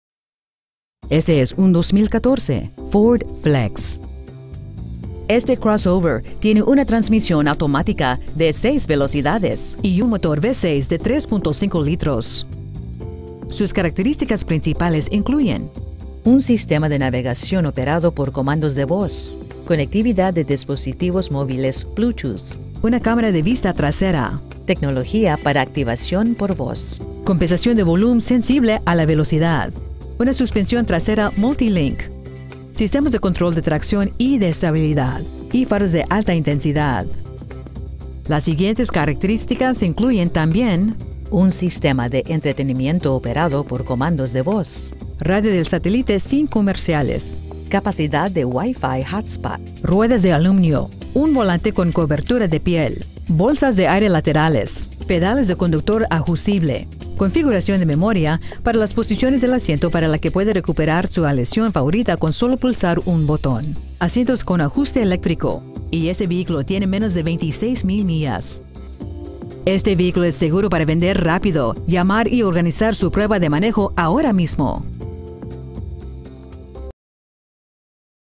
2014 Ford Flex Para La Venta en Palm Beach FL
Haz clic aquí para ver el precio Hemos tenido el honor de servir a la zona de Lake Worth fl , le prometemos que su experiencia superará sus expectativas ! Year : 2014 Make : Ford Model : Flex Engine : Motor con 6-Cil de 3.5L Trans . : Transmisión Automática de 6 Velocidades Millaje : 25,058 Stock : 84764 Offleaseonly.com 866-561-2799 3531 Lake Worth Rd Lake Worth , fl 33461